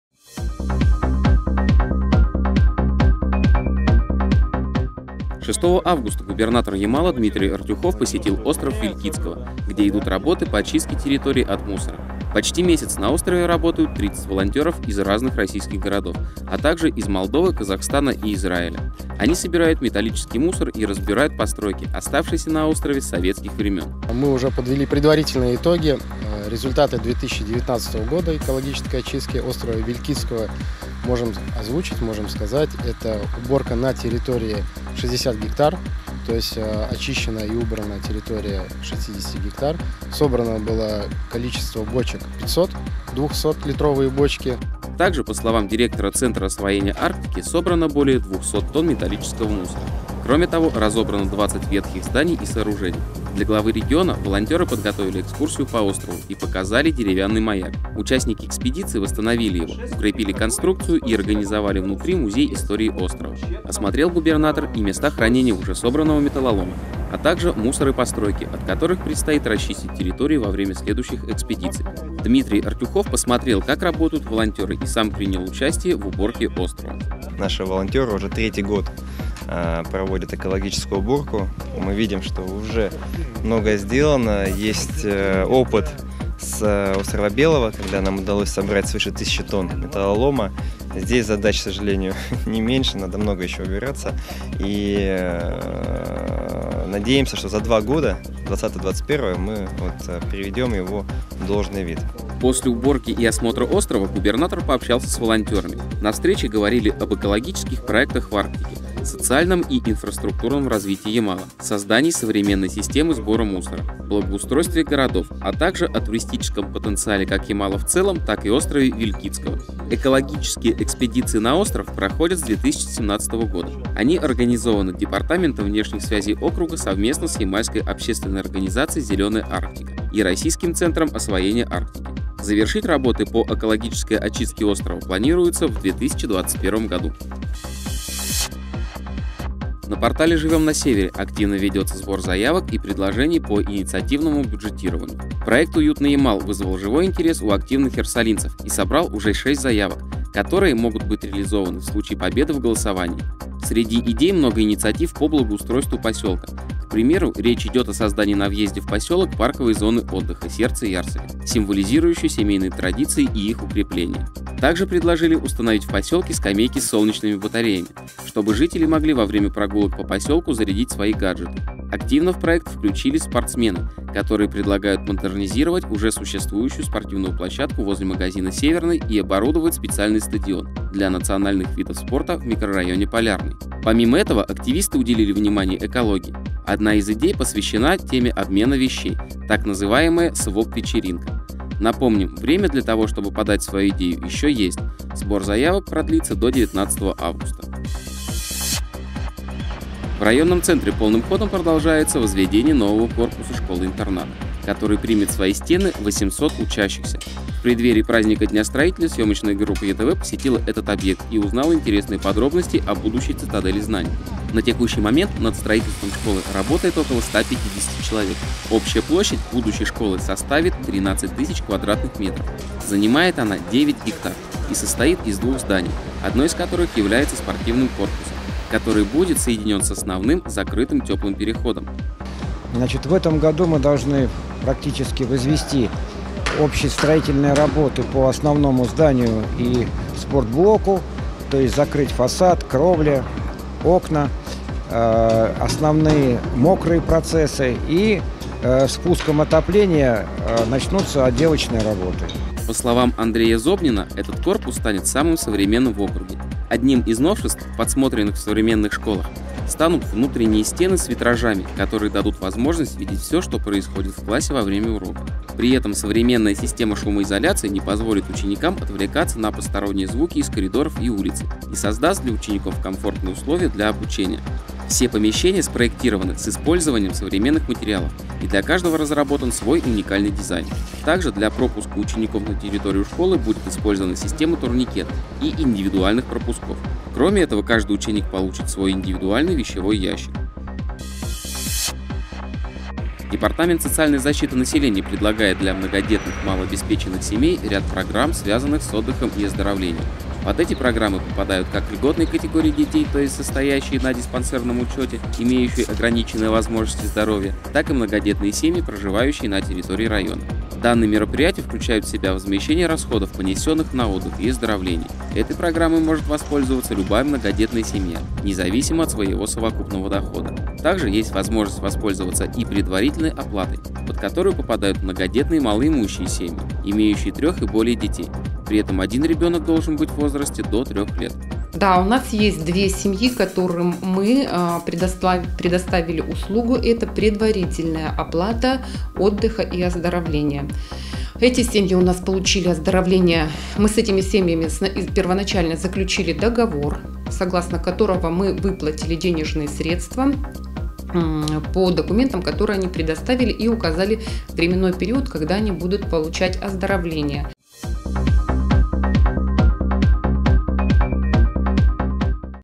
ВКУРСЕСОБЫТИЙ 7 августа 2019
- В Яр-Сале продолжается возведение нового современного корпуса школы-интерната.
- На портале «Живем на севере» активно ведется сбор заявок и предложений по инициативному бюджетированию.
- Департамент социальной защиты предлагает ряд программ для многодетных и малообеспеченных семей.
- Дмитрий Артюхов побывал на острове Вилькицкого и принял участие в экологической уборке Арктики.